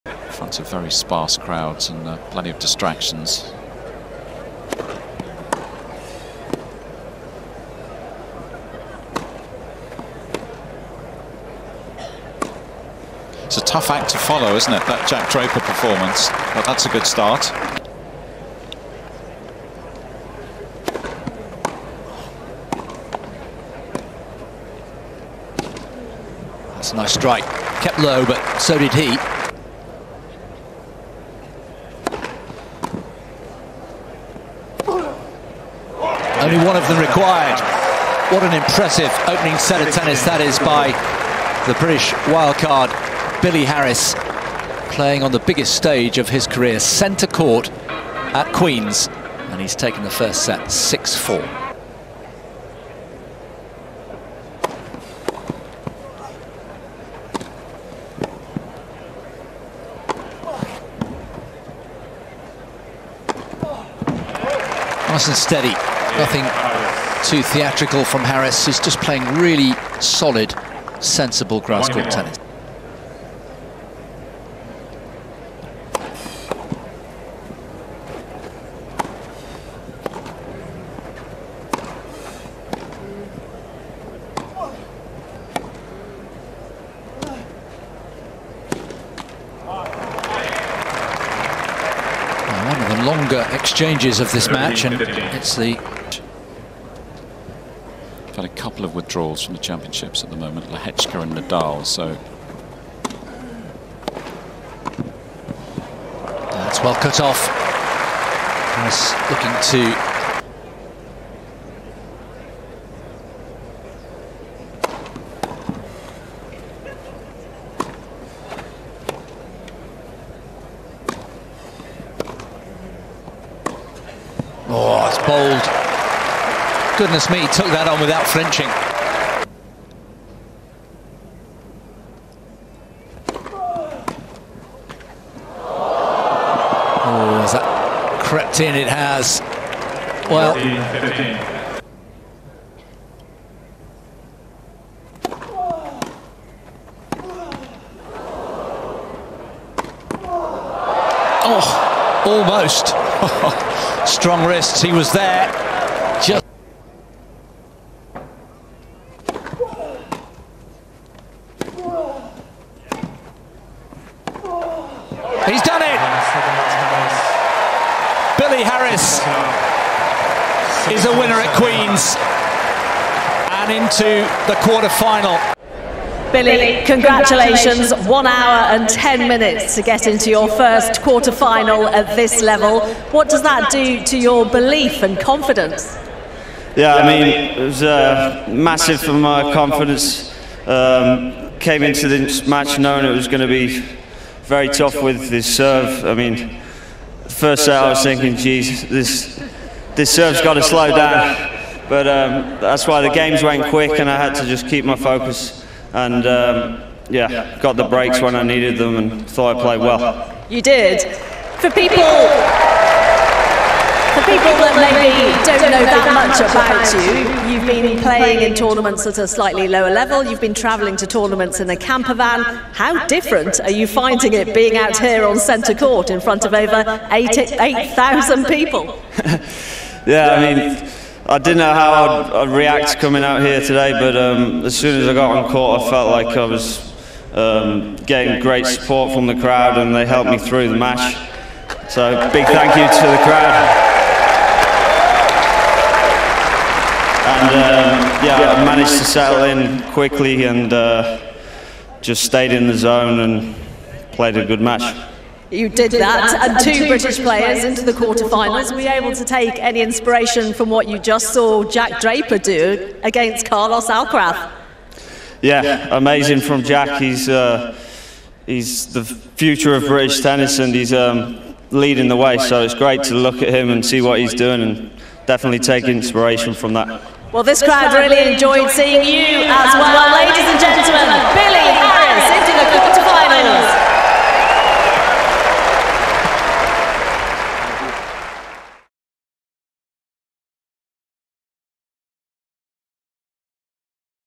Harris's First QUARTERS! Highlights - Billy Harris v Giovanni Perricard | cinch Championships 2024
Watch highlights as Brit Billy Harris achieves a new career milestone after making his first ATP 500 quarter-final at the cinch Championships – defeating qualifier Giovanni Mpetshi Perricard 6-4, 7-5 in just one hour and 10 minutes in London at the Queen's club.